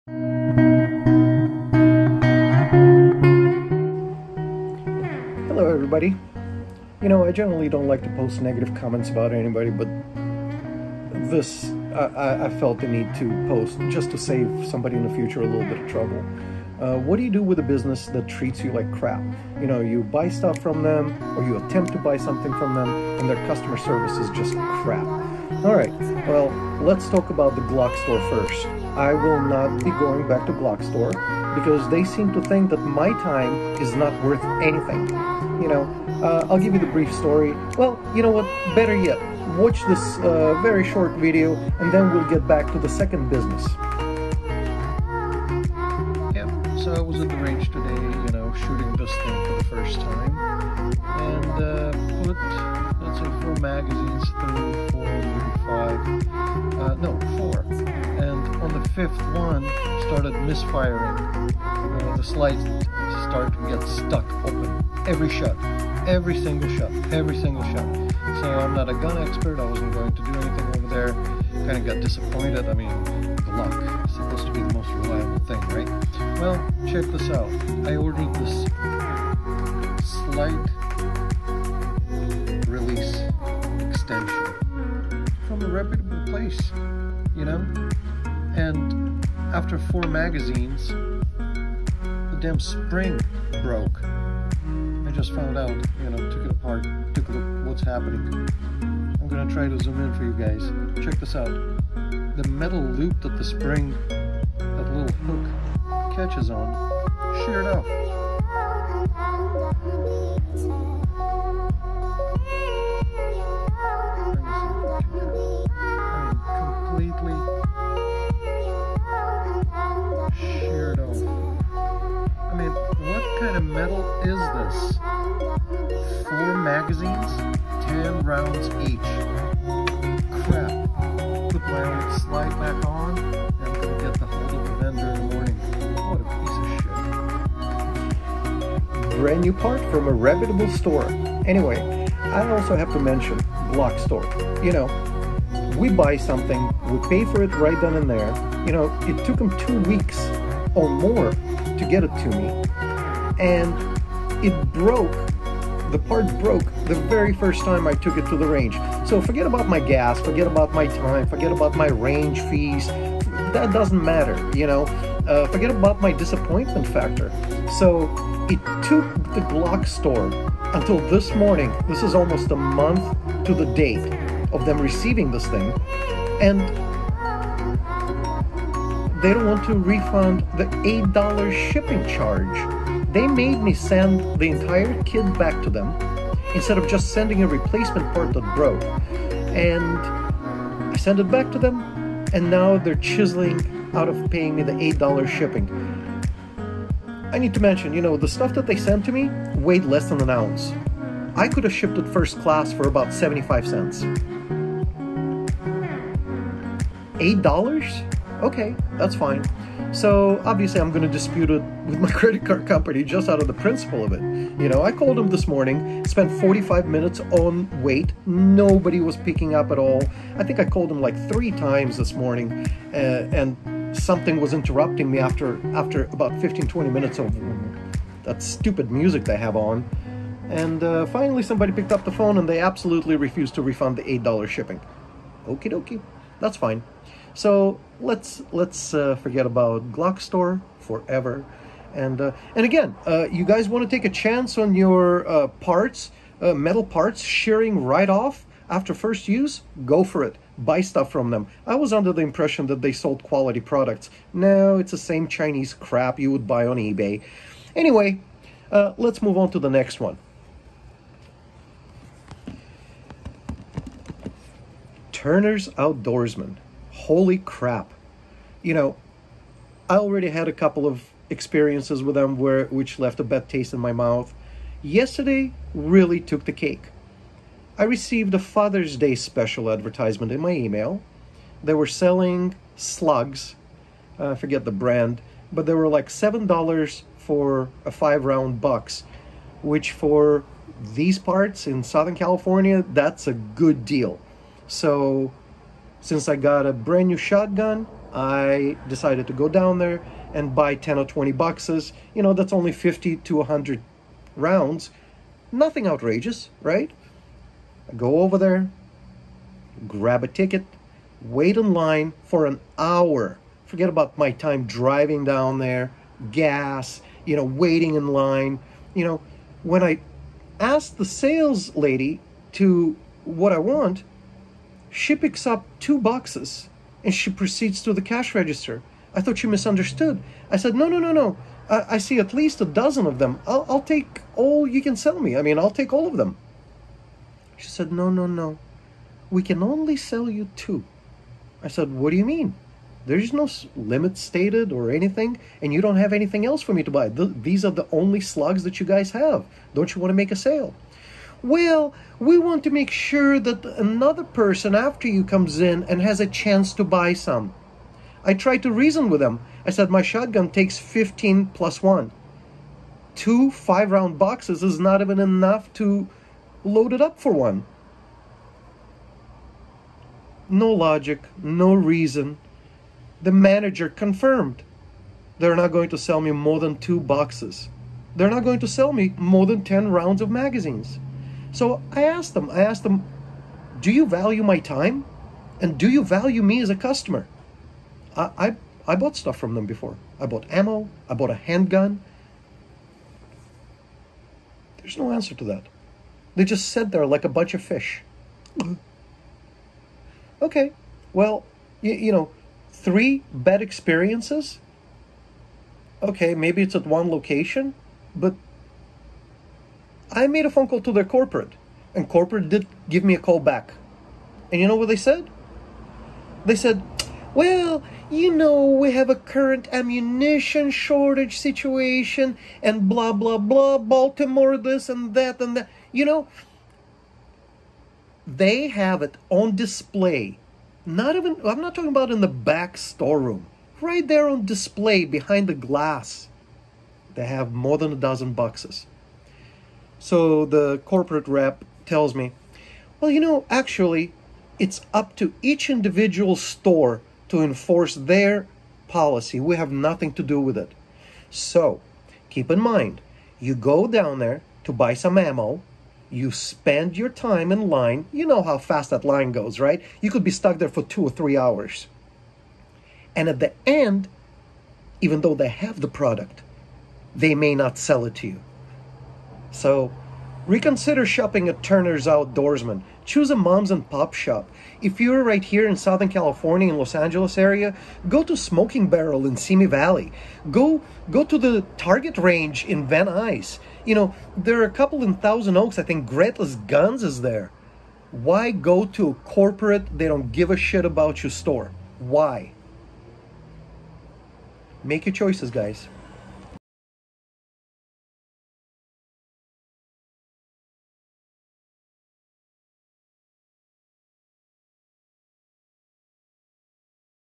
Watch this if you plan on shopping at The Glockstore or Turner’s Outdoorsman
Just a whole lot of suck from these two stores. My time and money will be better spent at the local mom and pop shops.